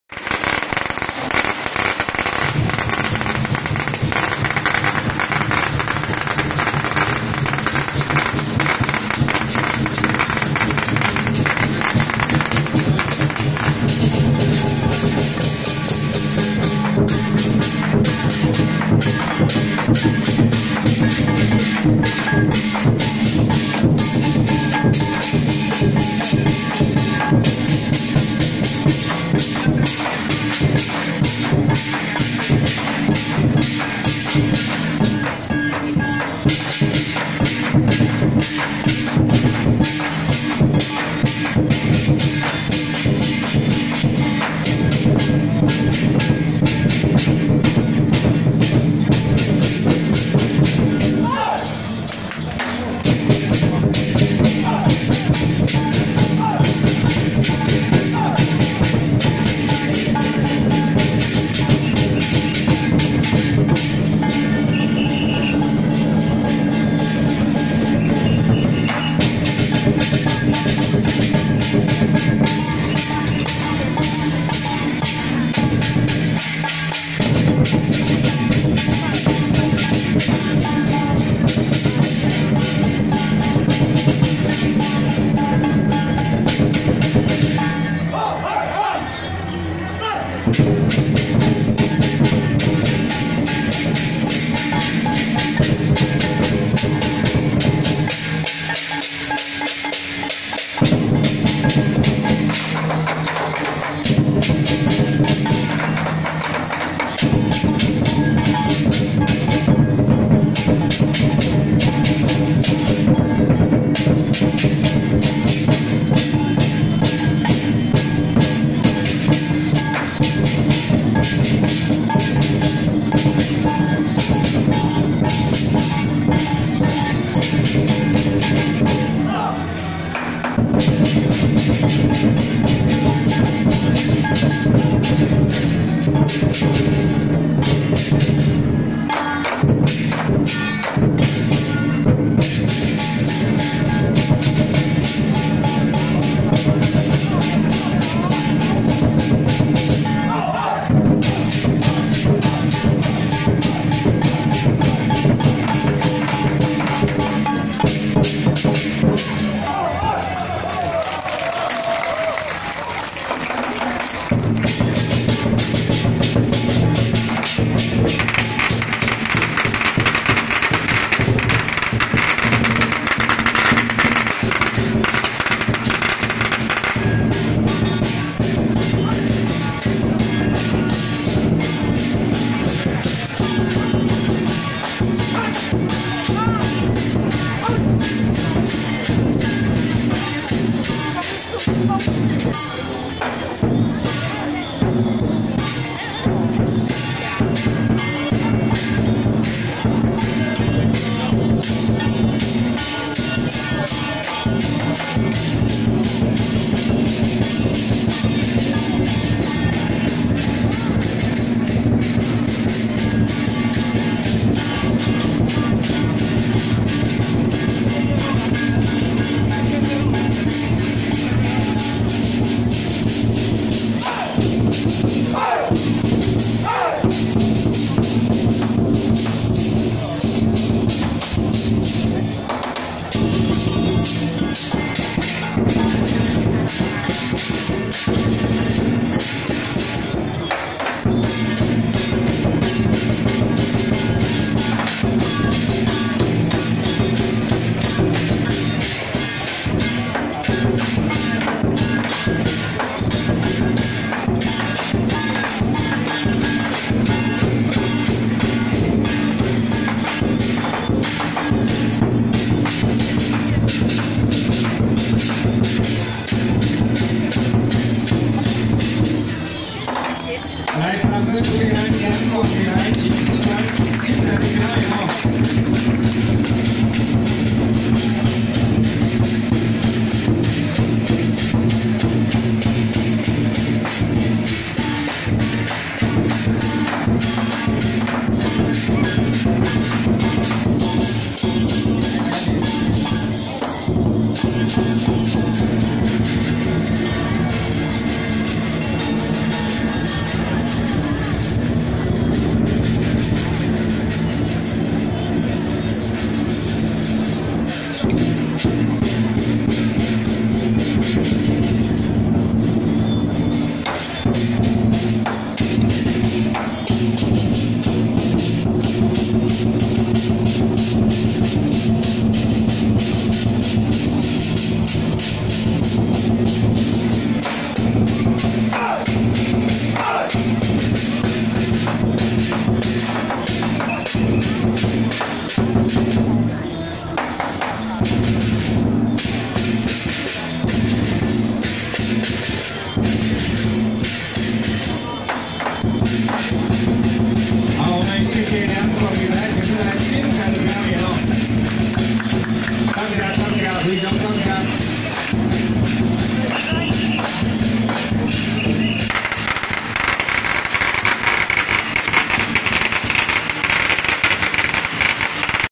107淡水福佑宮恭迎天上聖母祈安遶境【兩廣旅臺國術-戰鼓團＆醒獅團】
兩廣旅臺國術-戰鼓團＆醒獅團